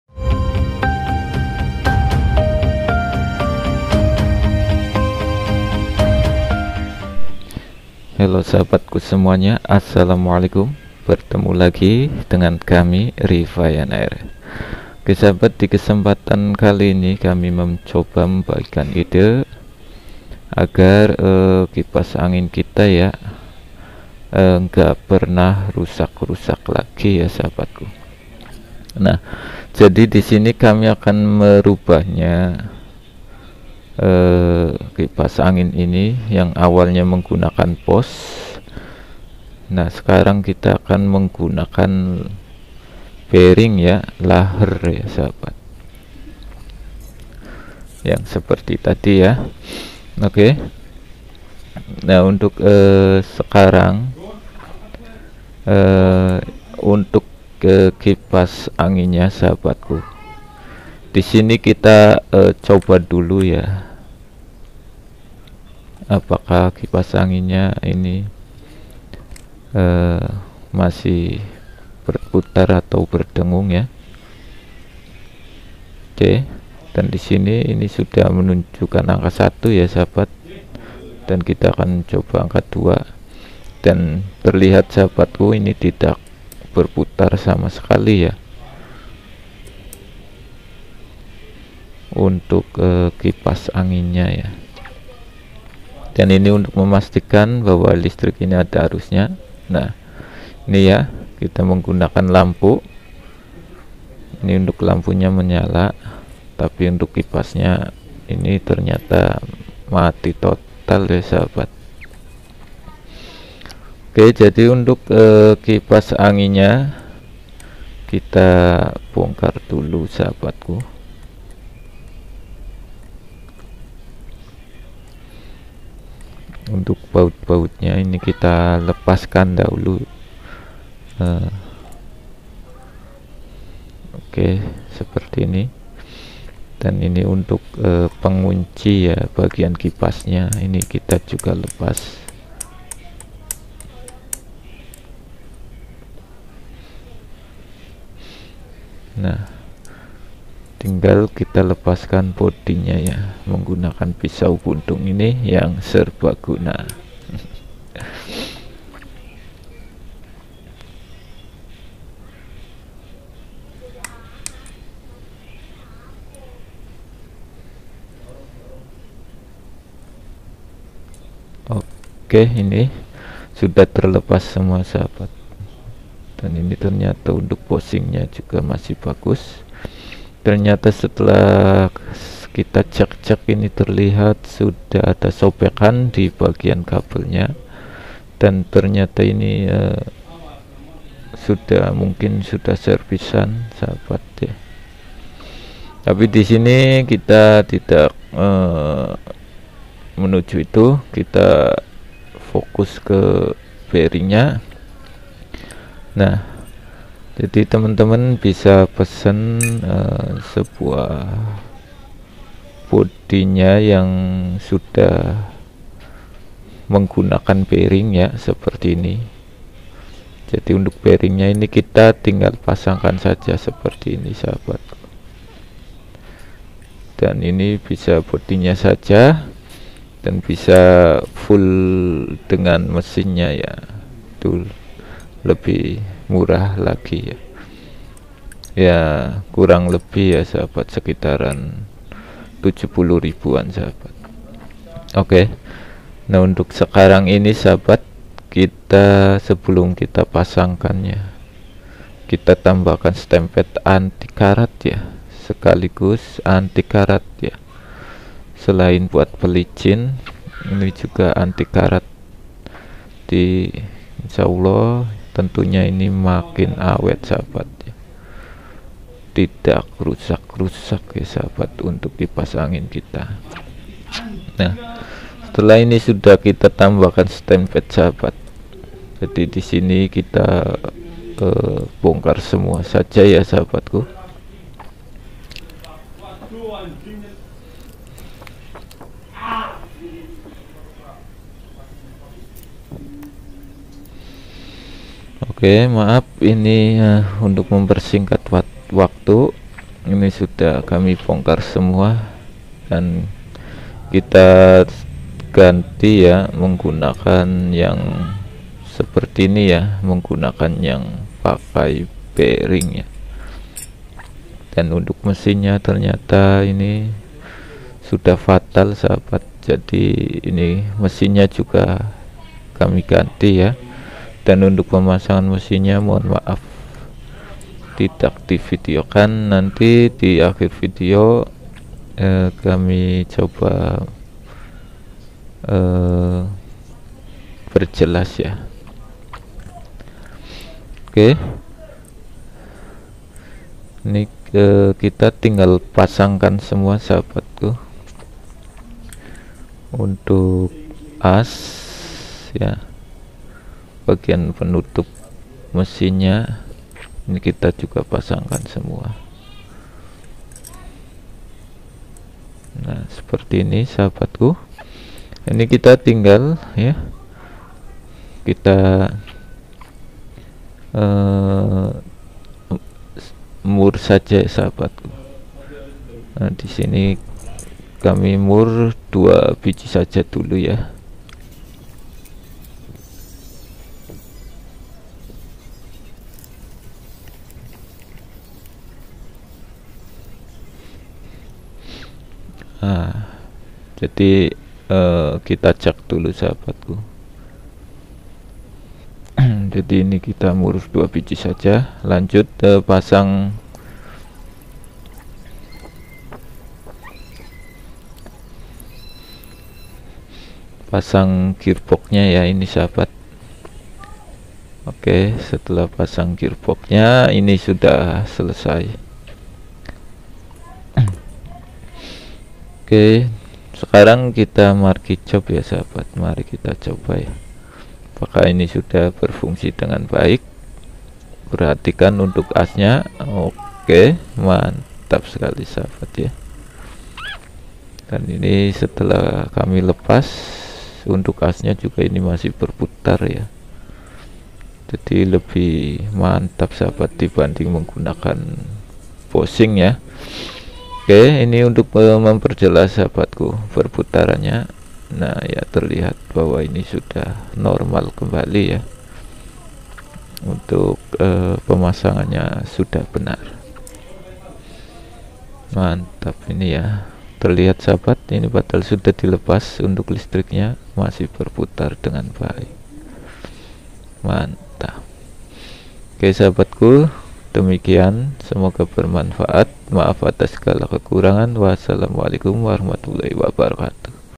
GARA GARA VIDEO INI BANYAK ORANG PAKAI CARA INI.Trik Membuat Kipas Angin Awet Bertahun Tahun !!!
TERNYATA BEGINI CARANYA KIPAS ANGIN GAKK PERNAH RUSAK !!!
    Oke jadi di kesempatan x ini kami coba bagikan sebuah ide kreatif yg unik dan tentunya bermanfaat,,
 yaitu tentang cara memperbaiki kipas angin agar awet,cara agar kipas angin gak pernah rusak,
dengan cara ini otomatis kita dapat hemat biaya dan kita gak lagi beli beli kipas angin lagi 🤑 ,,,
OK sahabat ku semoga ide kreatif ini bermanfaat. ,,
trima kasiiih,,, maaf atas segala kekurangann.
👍👍👍😍😍😍 ,,,,SELAMAT MENCOBAA,,,. ,,,,🥰🥰🥰

 Lokasi syuting di rumah ibu tercinta ,,,  bermodal alat ala kadarnya , yaitu pakai pipa pvc/paralon bekas, tempat dudukan hp dpt pinjam, pencahayaan dari lampu rusak juga yg sudah kami perbaiki ,, dan HP Redmi A8 pro   ,,,,, 🤗🤗